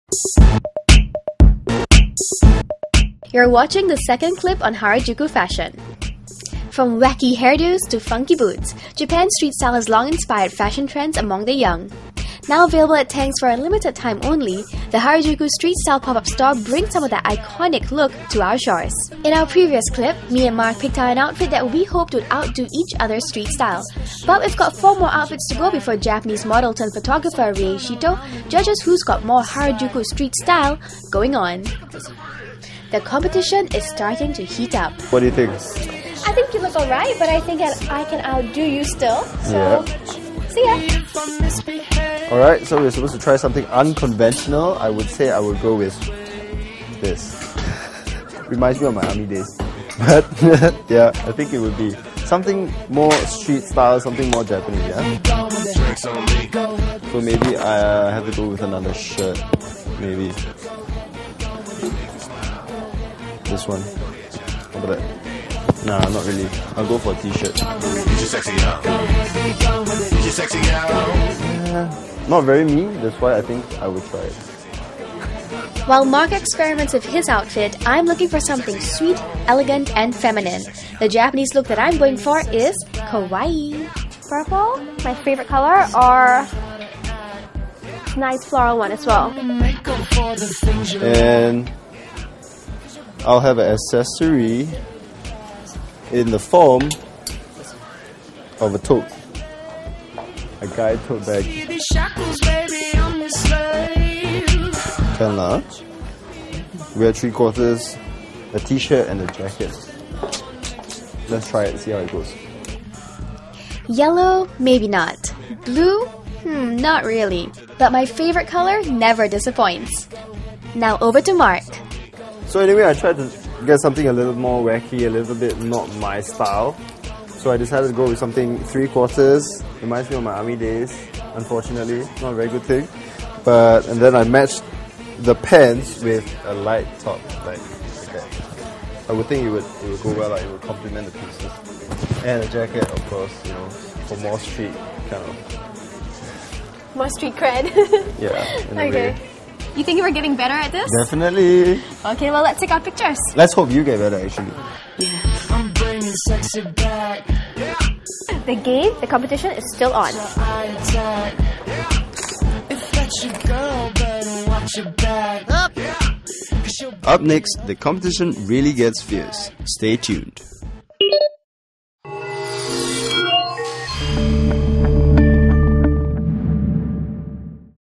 So Kawaii! (Harajuku Fashion Pt 2)
Mark and Eva put their fashion sense to the test, and pick out an outfit that will impress Japanese model turn photographer, Rei Shito. Who do you think has better street style? Click on the Poll button to take part!